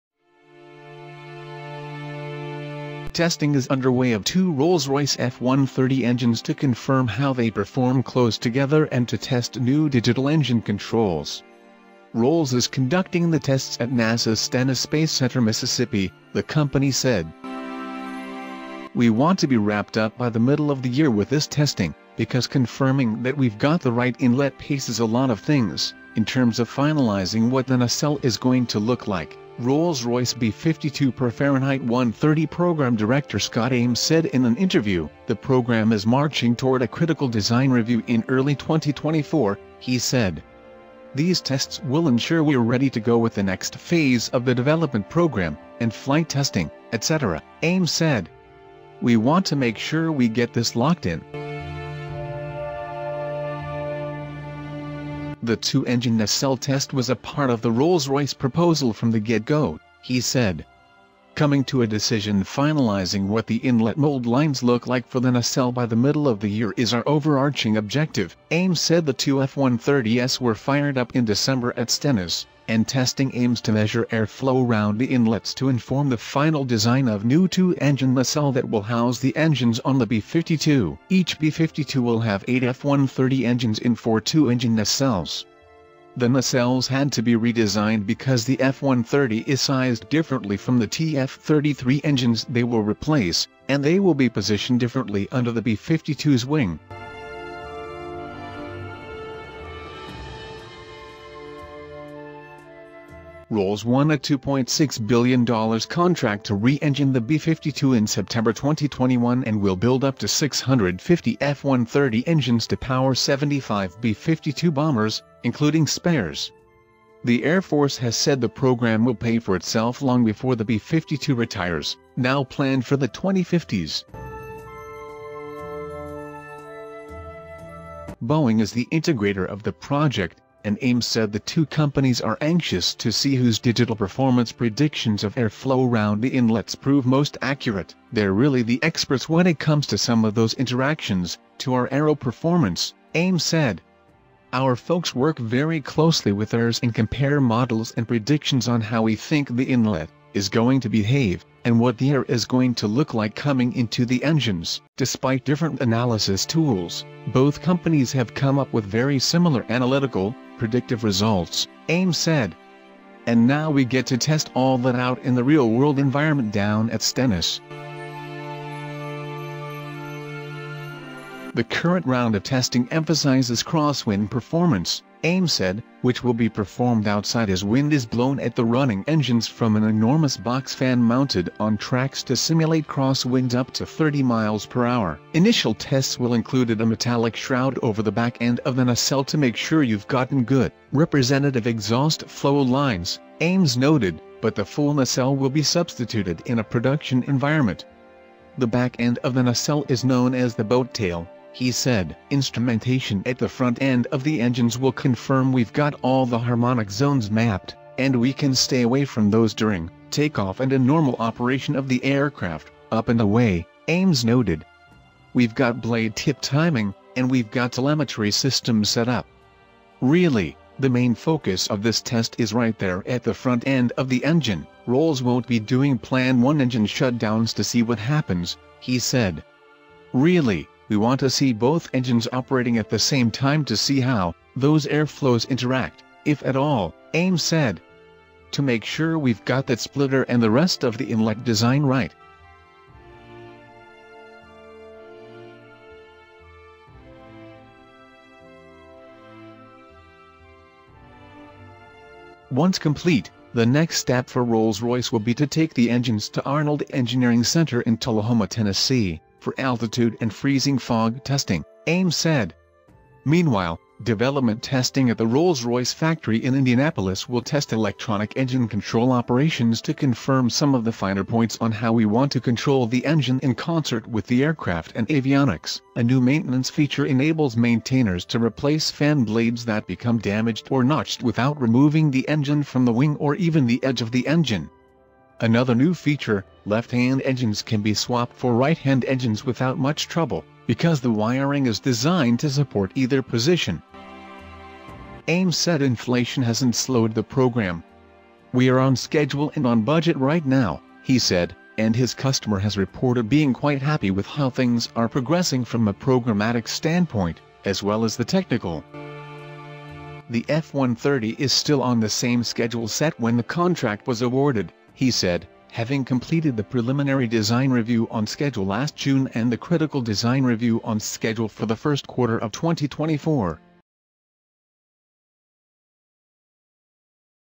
Roll-Royce | The Rolls-Royce F130 engine for the B-52 begins its sophistication test
Testing is underway of two Rolls-Royce F130 engines to confirm how they perform close together and to test new digital engine controls. Rolls is conducting the tests at NASA’s Stennis Space Center, Miss., the company said.